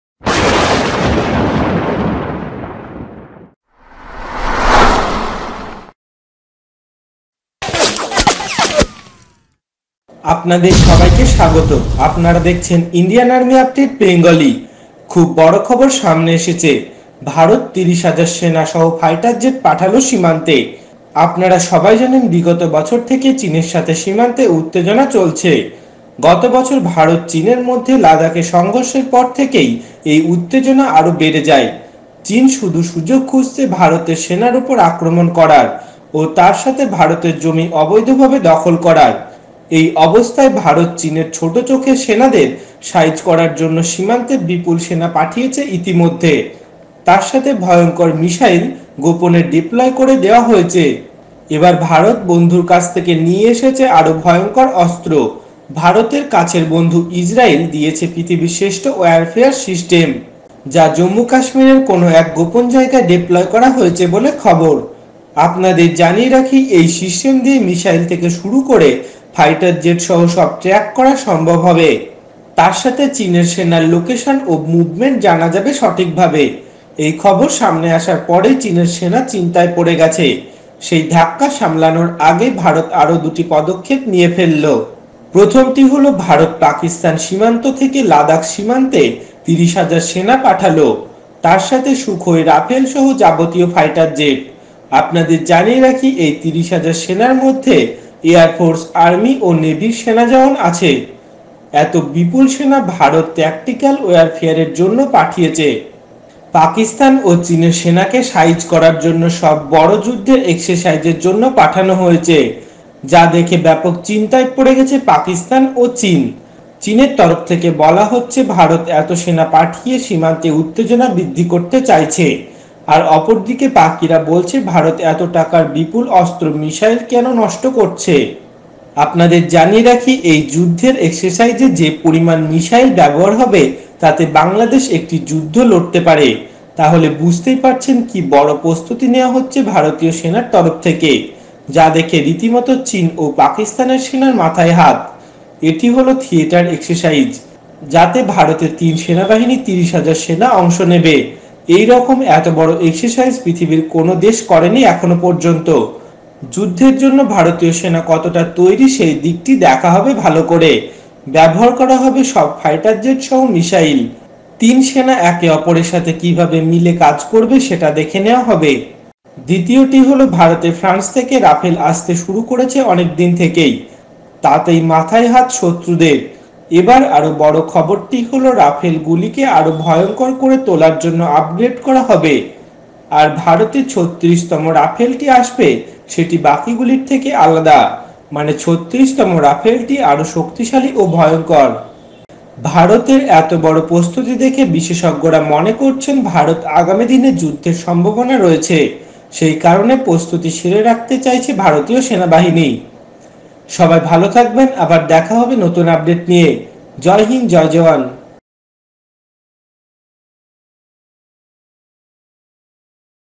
Update 234, Big News ভারত ৩০ হাজার সেনা সহ ফাইটারজেট পাঠালো LAC ওLOC তে | Indian Army update Bengali
Big News ভারত ৩০ হাজার সেনা সহ ফাইটারজেট পাঠালো LAC ওLOC তে | Indian Army update Bengali